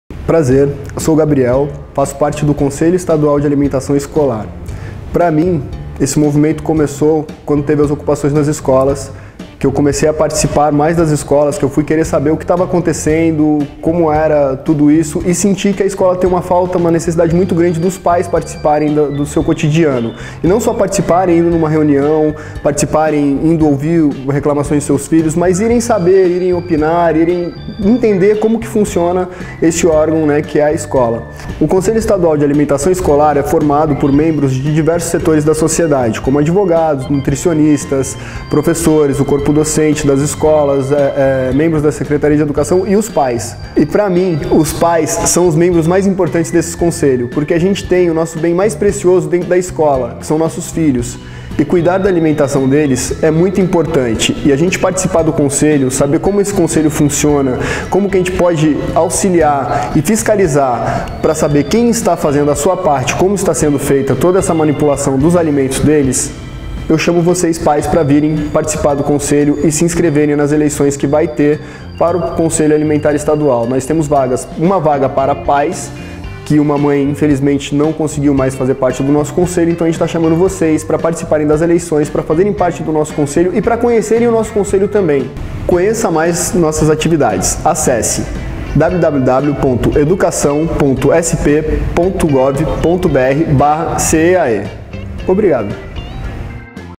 Conselho de Alimentação abre eleição para a categoria Pais de Alunos
As inscrições vão até 20/10 e a eleição para o Conselheiro Suplente será dia 27/10